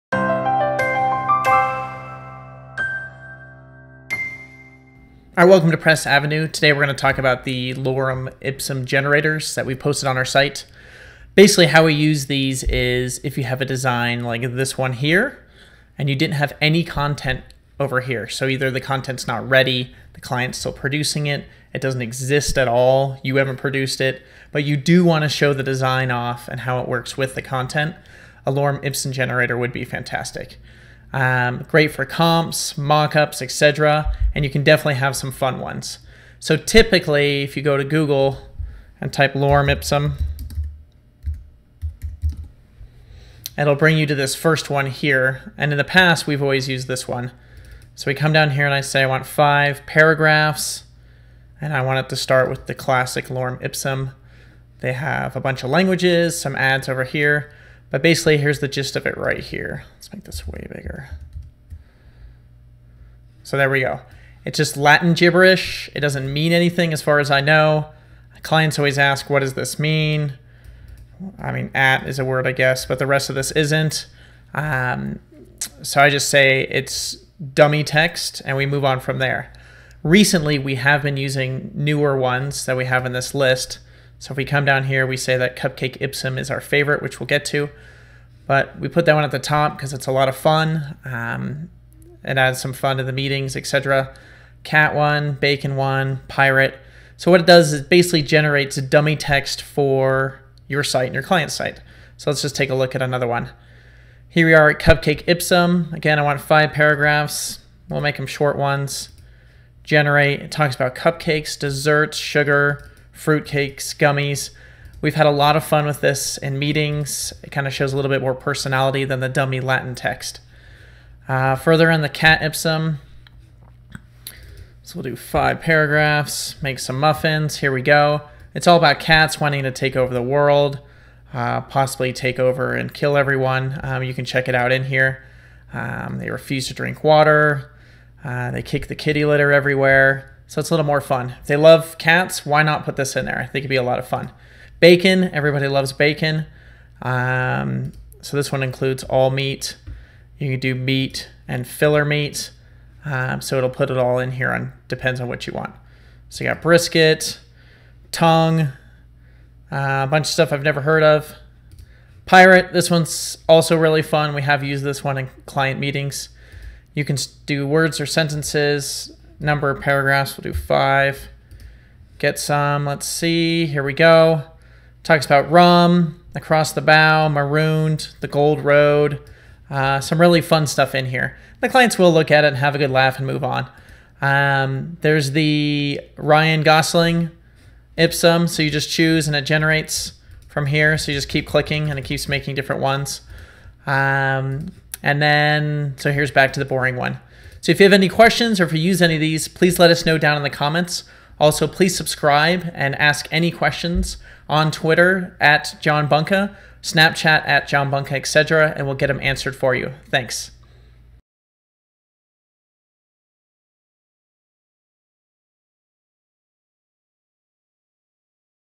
Lorem Ipsum Generators for Dummy Text in Websites – Press Avenue
Lorem Ipsum Generators for Dummy Text in Websites

We use Lorem Ipsum Generators to fill in content before the content/marketing copy is ready. This is great when developing a new website, pitching an idea or filling in content areas to see what the future text could or will look like.

Lorem Ipsum Generator. See the full blog post on Press Avenue